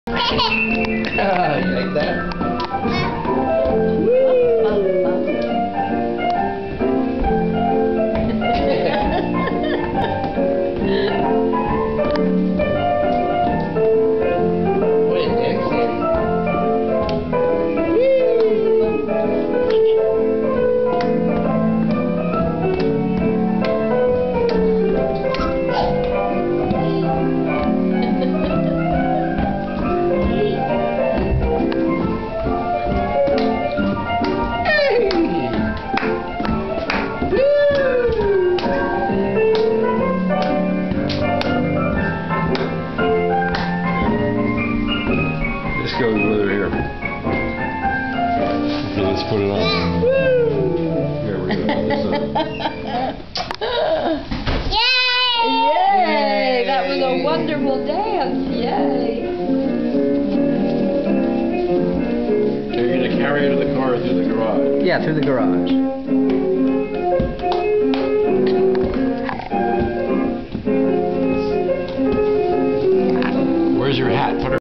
Uncle Aryeh Jazzes it up with Leigh
Watch Leigh smile as she whirls with Uncle Aryeh and Grandma Joan clap to the beat